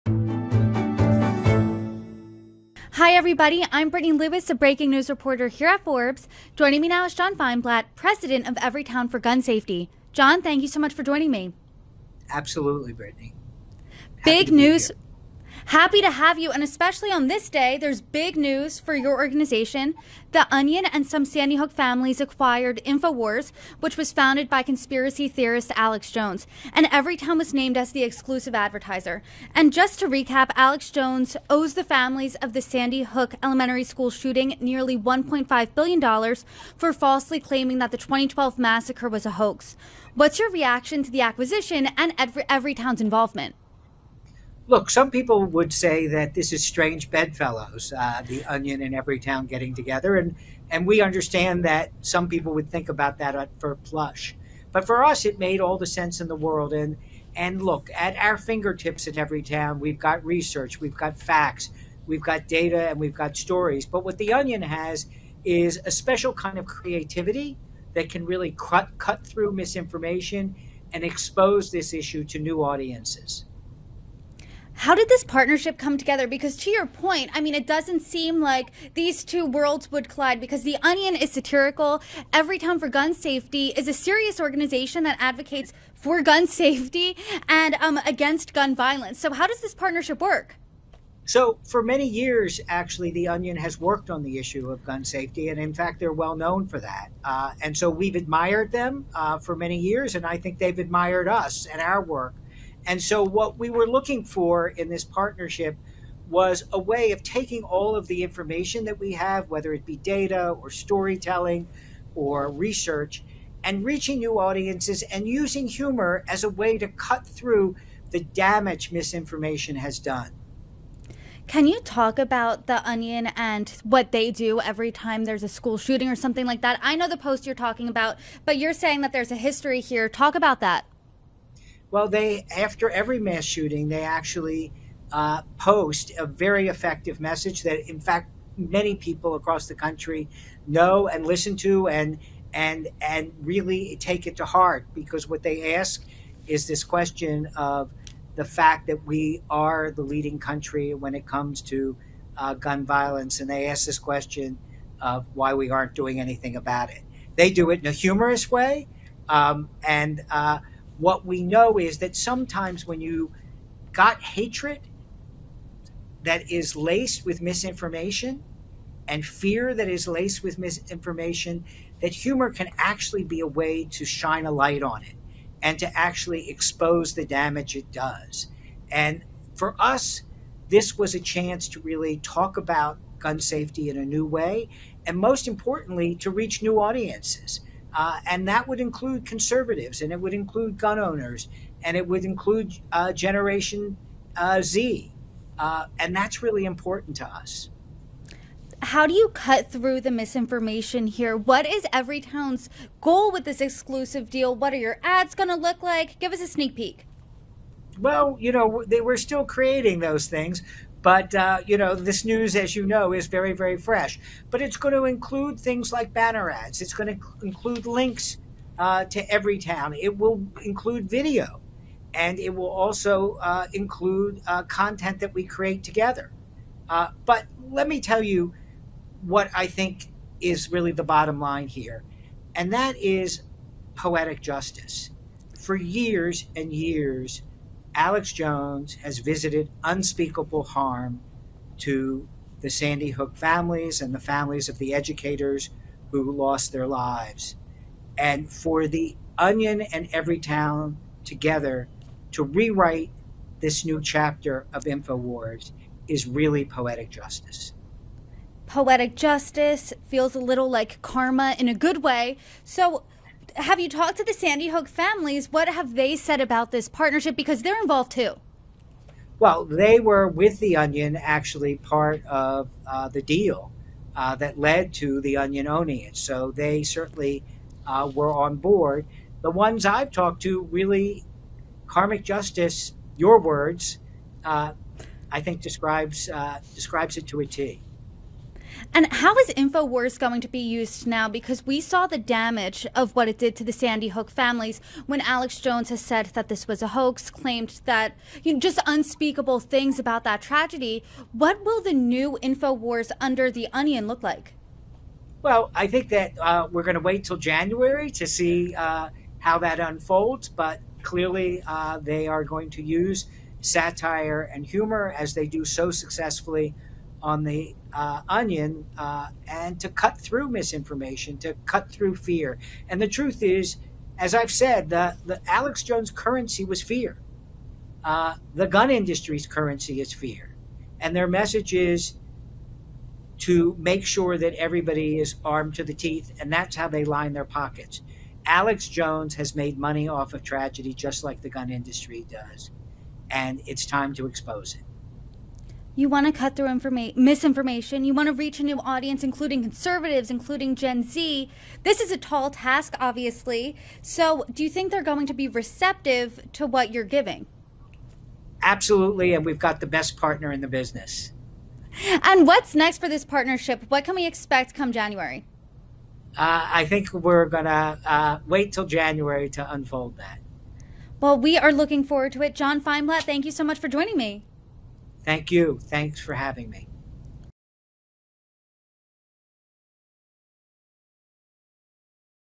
'Poetic Justice': Everytown President Reacts To Satire Site The Onion Buying Alex Jones' Infowars
The parent company of the satirical site The Onion acquired Infowars, a franchise founded by conspiracy theorist Alex Jones. Infowars was recently auctioned off to help Alex Jones pay the nearly 1.5 billion dollars he owes the families of the Sandy Hook Elementary school shooting victims after he falsely claimed that the 2012 massacre was a hoax. Everytown For Gun Safety was named as the exclusive advertiser on the new platform. Everytown’s President John Feinblatt joins “Forbes Newsroom” to discuss the partnership.